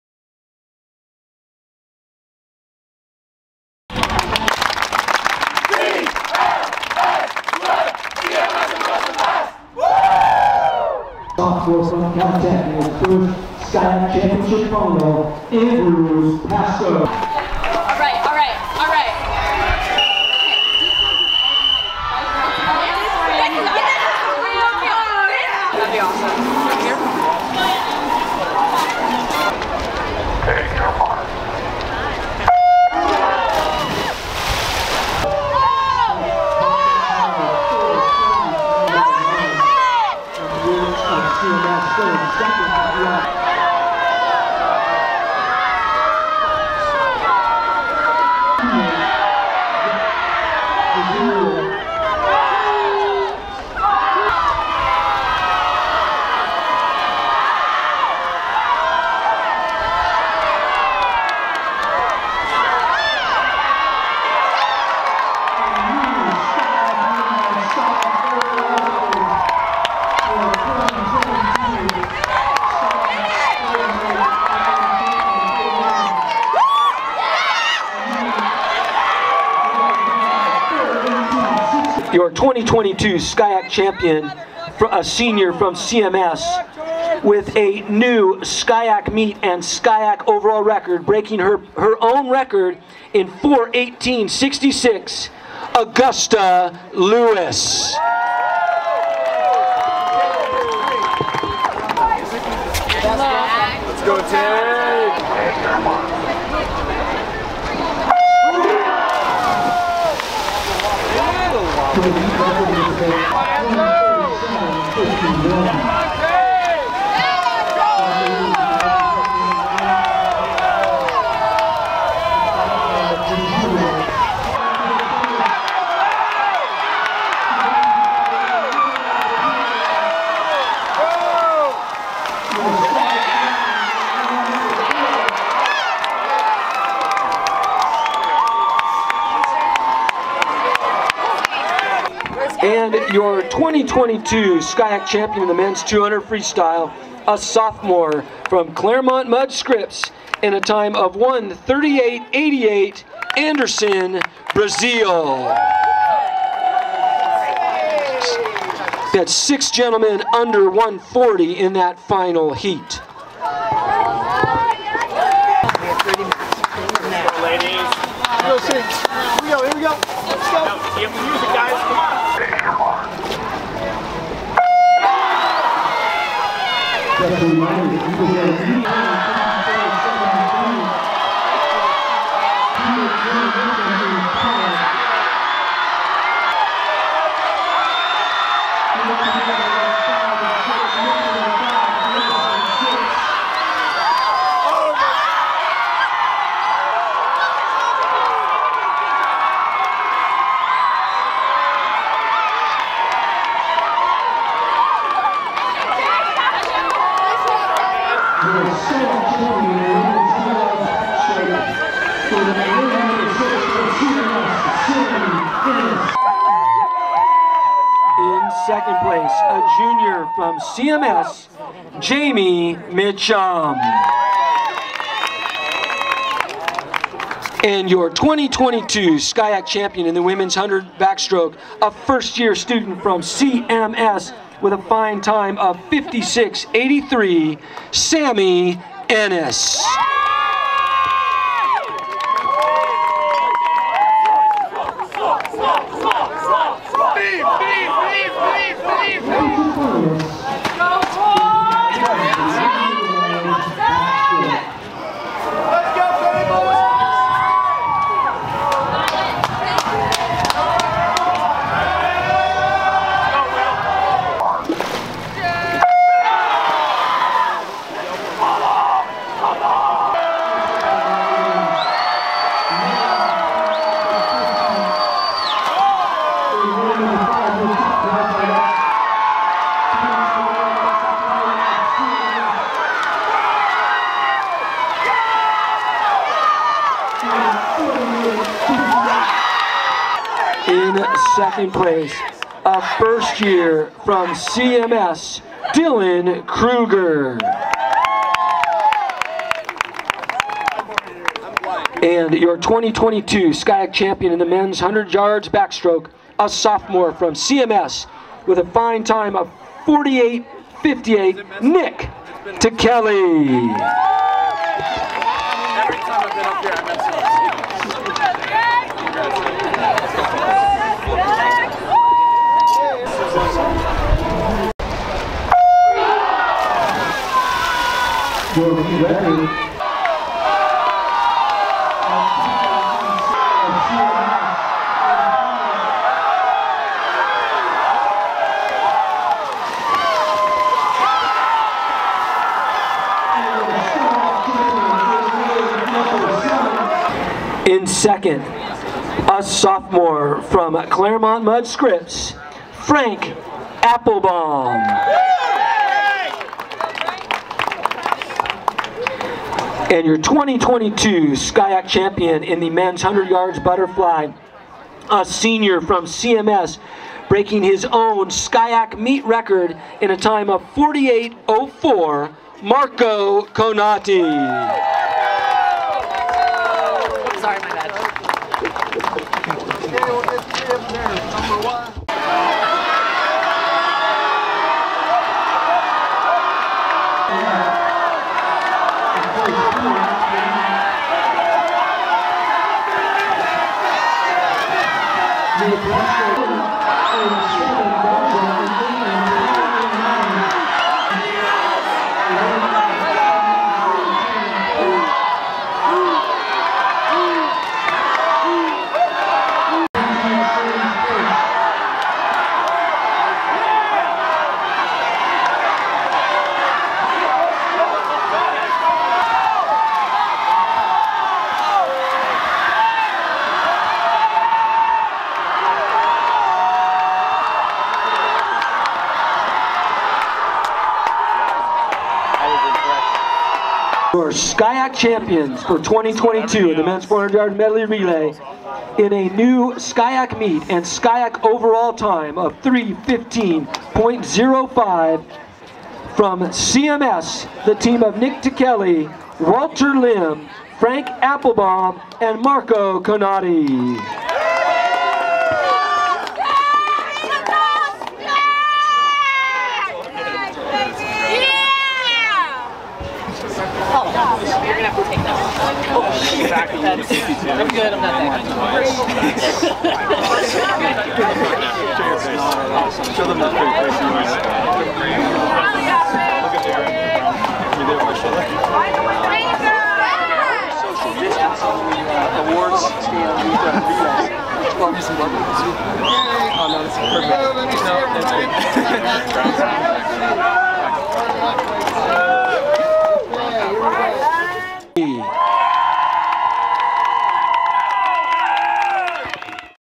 Swim & Dive Highlights - SCIAC Day Three (2/18/22)
The CMS Swimming and Diving teams won six of the 12 events held on the third day of the SCIAC Championships on Friday. Here's a look at some of the action.